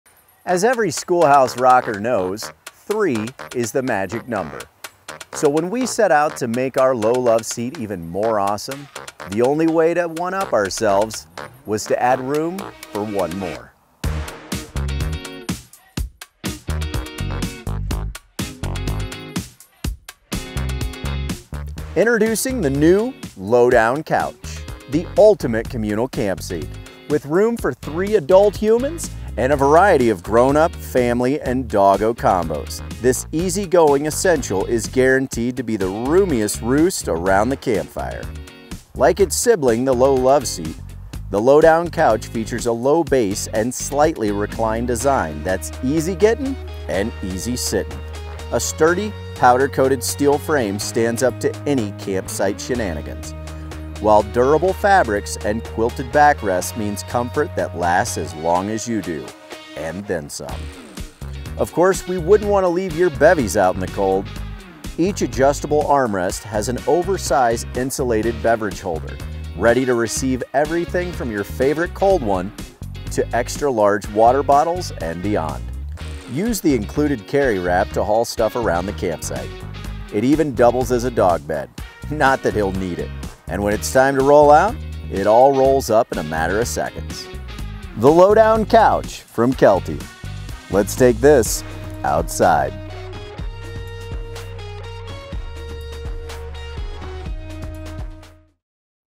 Kelty Lowdown Couch
THE ULTIMATE CAMP COUCH

We love our Love Seat, but we may have just one-upped ourselves. This 3-person (or 2-person, 1-dog) camp couch is everything you needed and more. Like our iconic Low Loveseat, the Lowdown Couch is built with a lower base and a relaxed, slightly reclined design so you’re getting your fill of chill without towering over the campfire. Three is a magic number, after all.